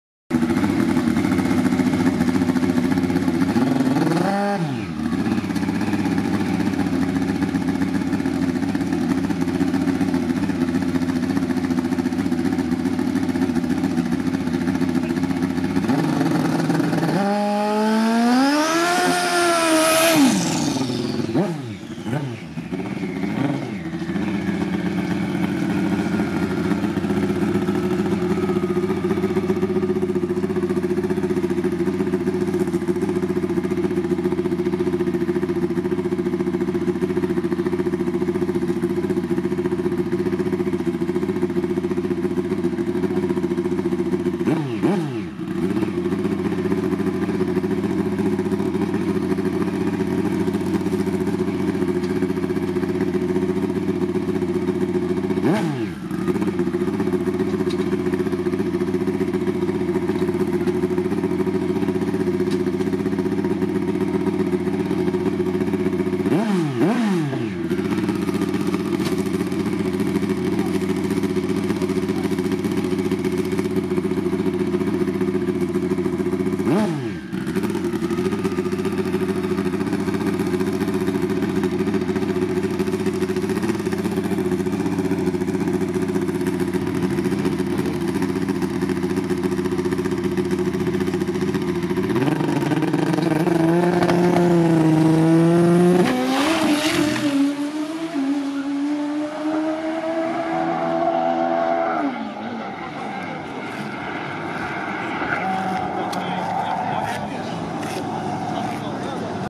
July 2020 RWYB Run 13
04th July 2020

Finally after the long 90 day break due to the Covid-19 pandemic situation, HAl Far Raceway has re-opened its doors. Tonight was the 3rd scheduled RWYB event for the MDRA 2020 calender.